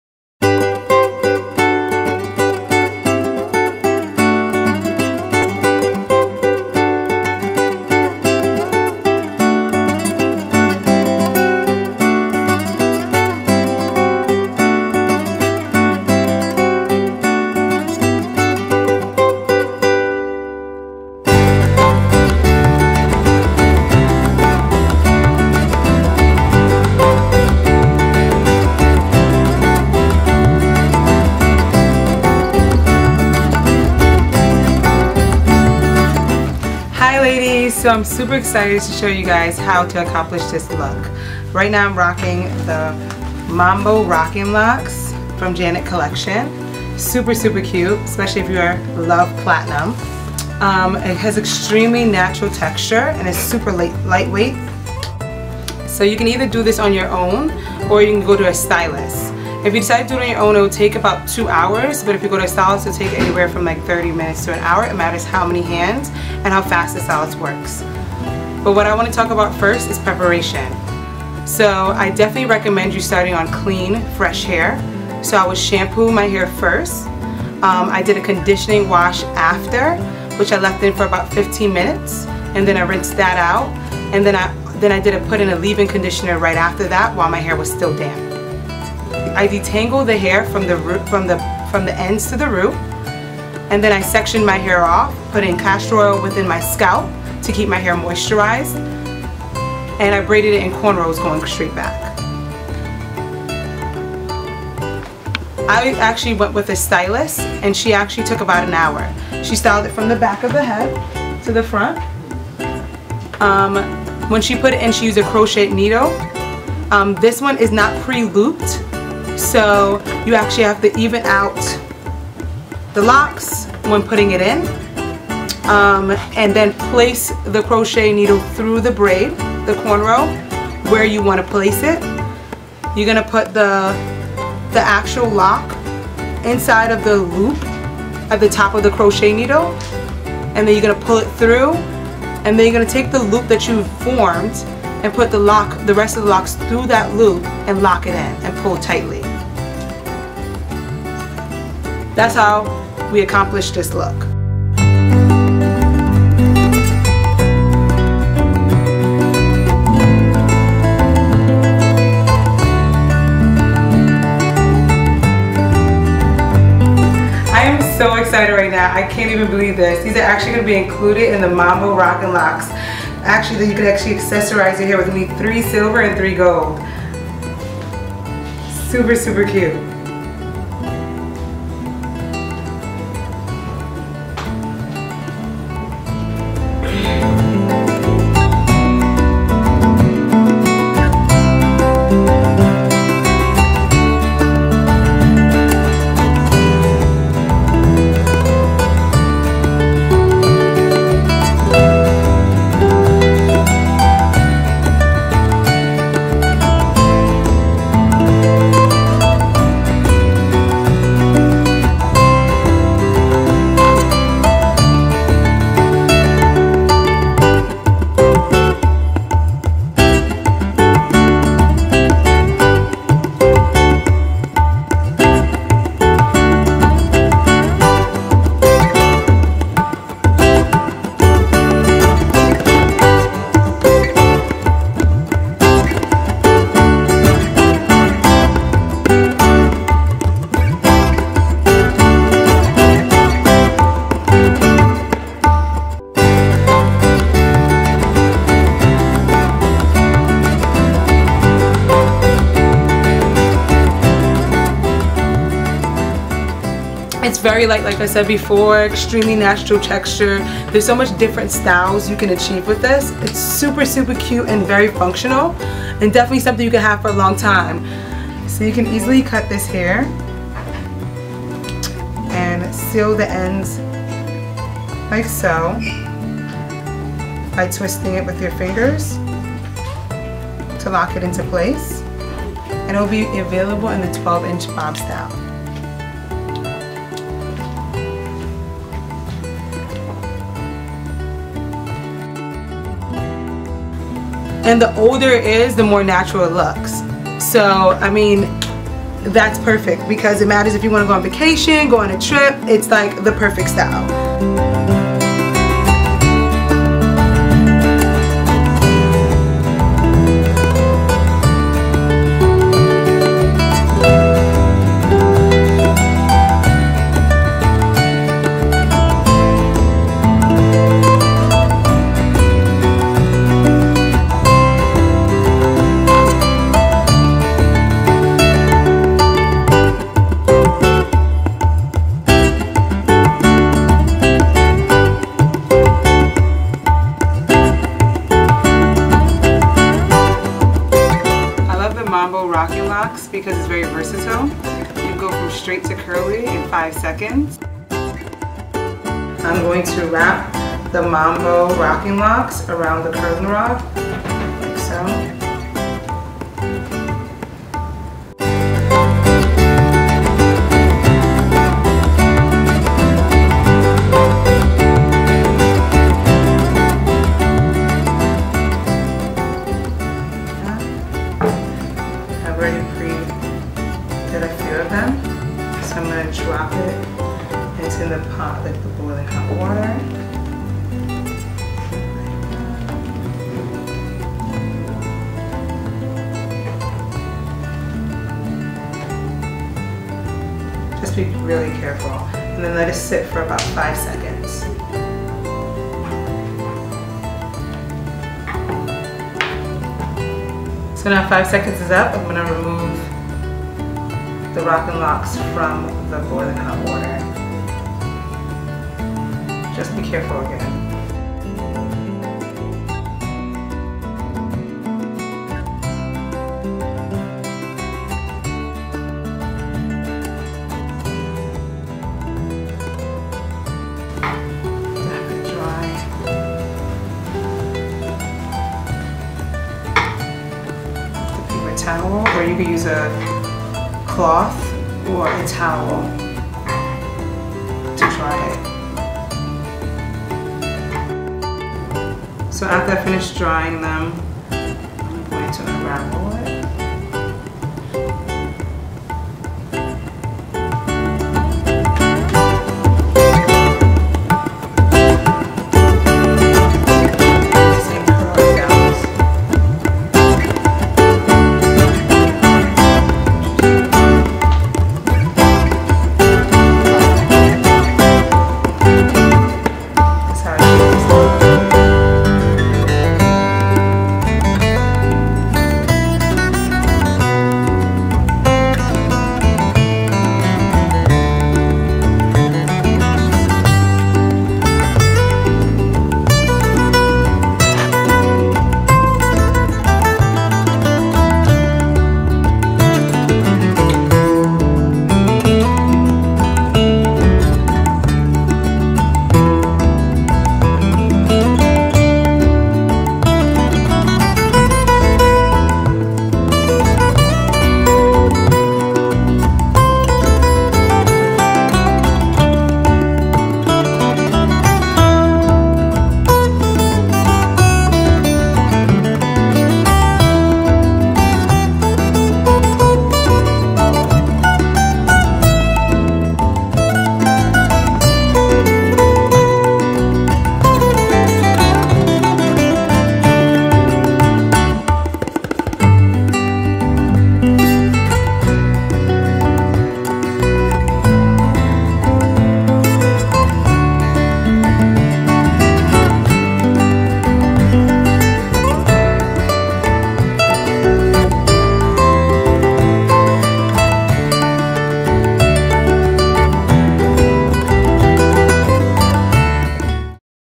MAMBO ROCKIN' LOCS TUTORIAL
Introducing our Mambo Rockin' Locs.
Extremely Natural Texture with light weight.
Look so natural and beautiful texture !
"The hair many influencers are in love with"
100% Hane-made locs